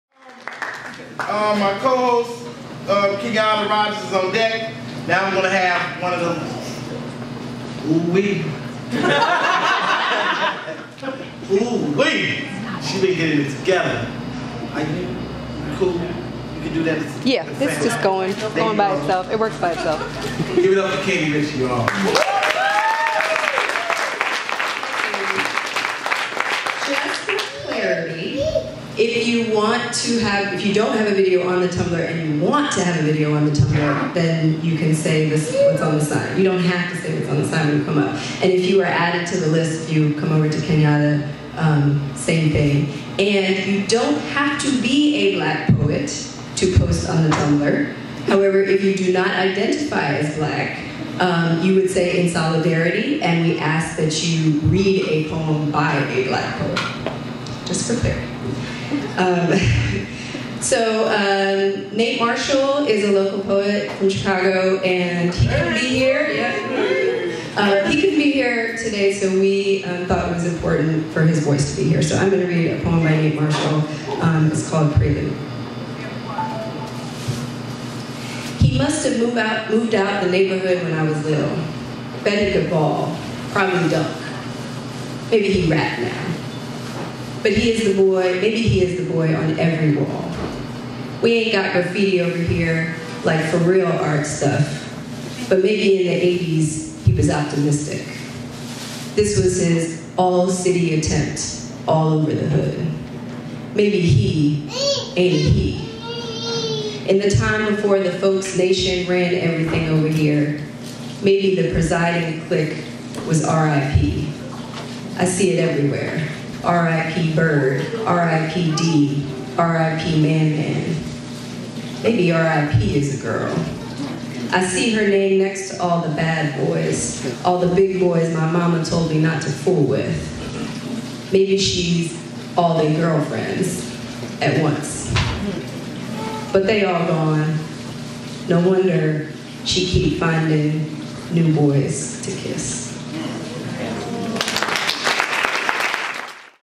Katy Richey reads "Prelude" by Nate Marshall
BlackPoetsSpeakOut Chicago